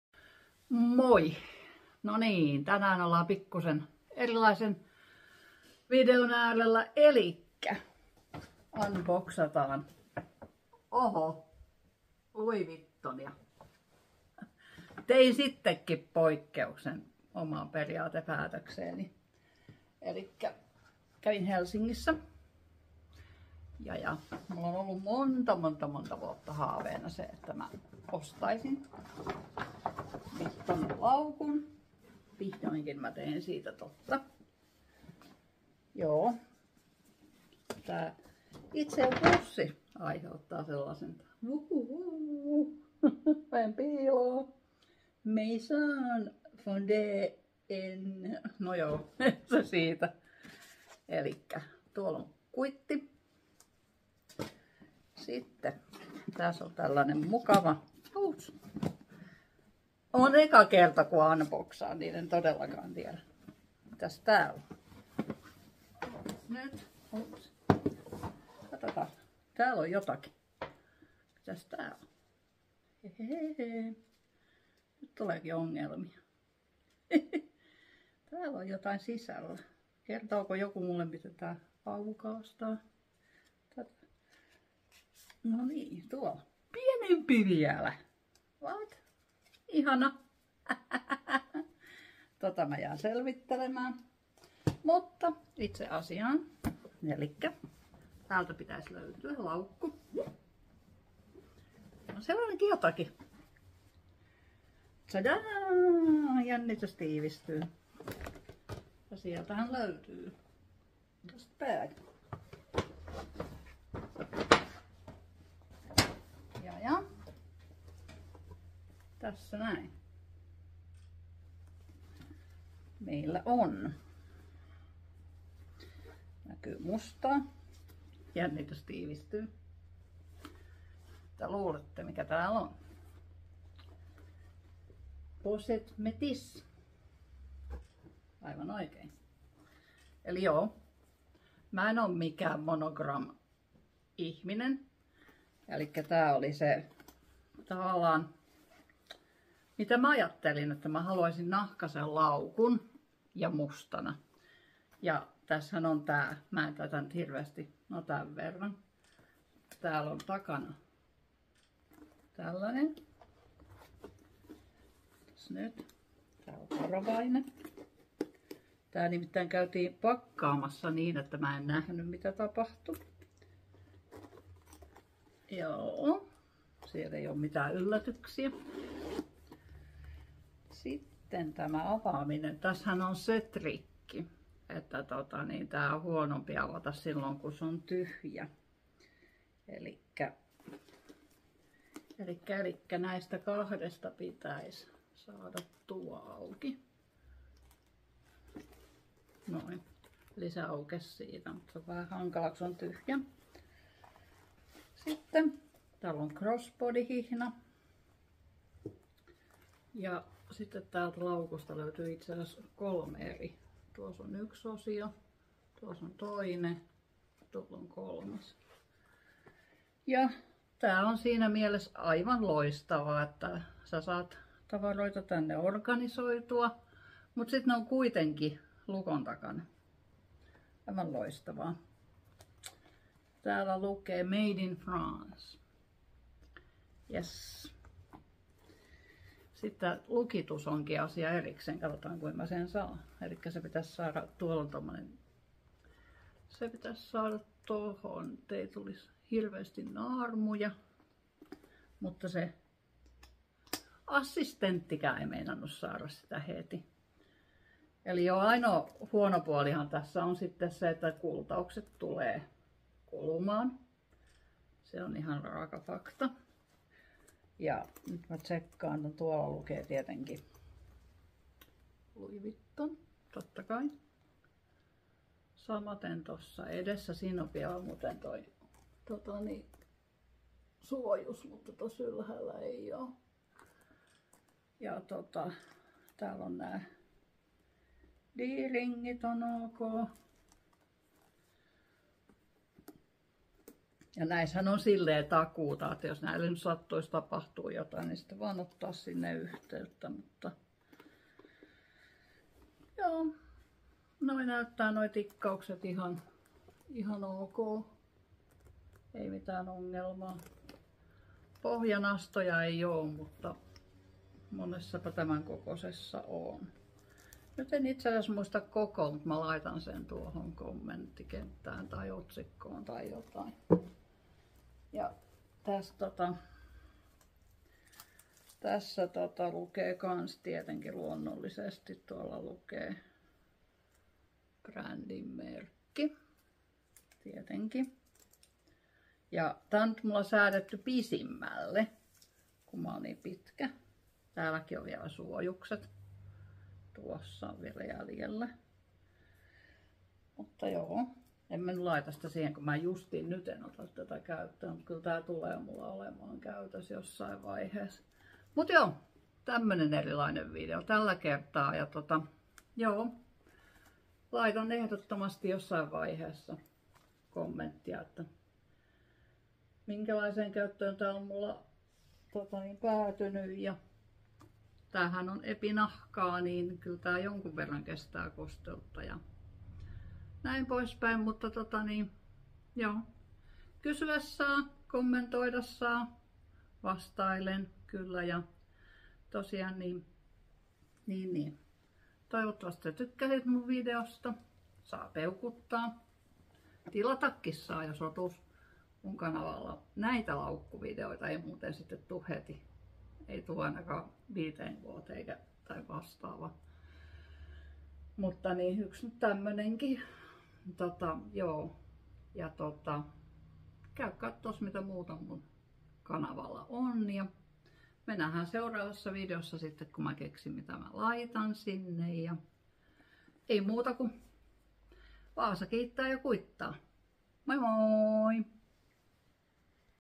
Jännittynyt fiilis avata omavalintainen ensimmäinen Louis Vuitton nahkalaukku. Pitkä odotus on ohi
Videossa paljastan ainoan LV laukkuni. Mitään varsinaista kokoa sillä ei ole.  Laukku oli lahja. Ihana ja yllättäväkin lahja, jonka muistan kyllä ikuisesti.

lv gift box
lv crossbodybag